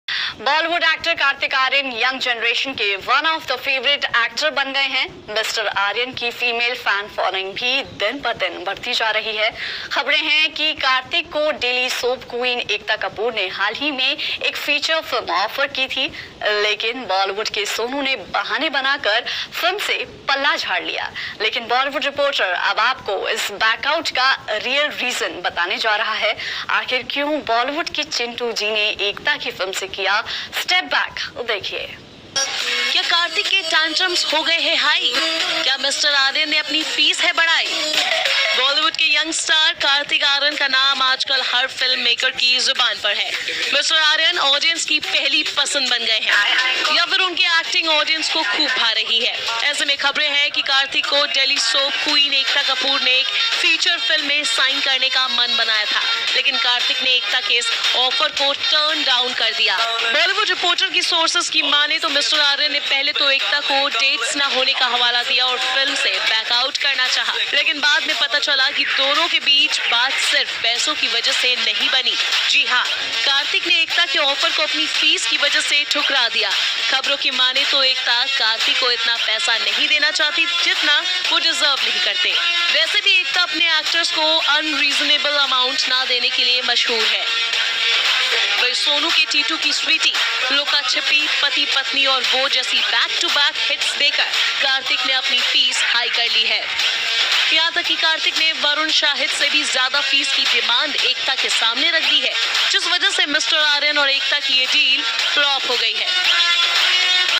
Kartik Aryan Reject Ekta Kapoor Film Offer
Katrina Aryan refused Ekta Kapoor Film.
Kartik Aryan
Ekta Kapoor
Bollywood news
Bollywood movies Kartik Aryan movies